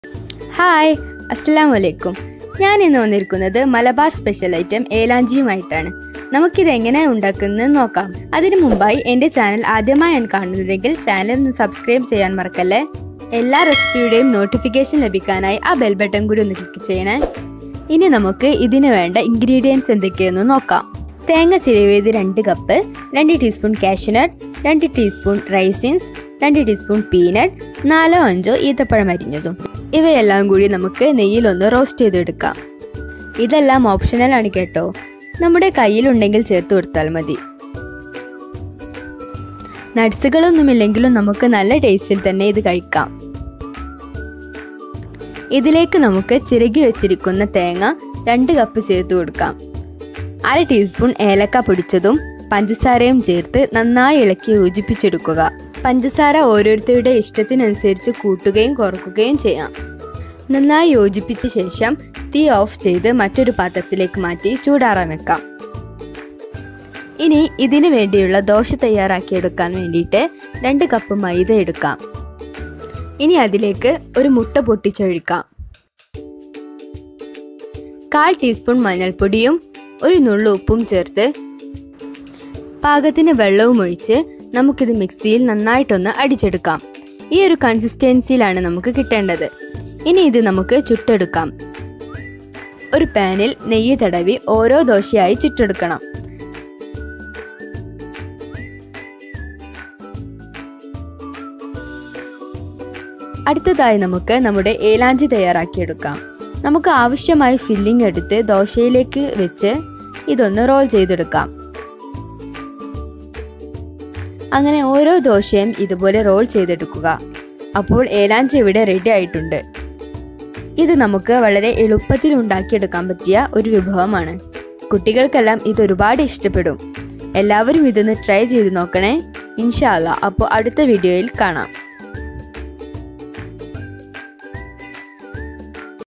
Elanchi/ഏലാഞ്ചി മലബാർ സ്‌പെഷ്യൽ ഈസി സ്നാക്‌സ്/Taste the best _by sana
ingredients:

നട്സുകൾ ഏതുമാവാം
തേങ്ങചിരകിയത്_2 cup
പഞ്ചസാര_ കാൽ cup
നെയ്യ്_2 tspn
ഏലക്ക പൊടിച്ചത്_അര tspn
ഉപ്പ് ആവിശ്യത്തിന്

ദോശക്ക് വേണ്ട ingredients:

മൈദ_2 cup
മുട്ട_1
ഉപ്പ് ആവിശ്യത്തിന്
മഞ്ഞൾപൊടി_കാൽ tspn
ആവിശ്യത്തിന് വെള്ളം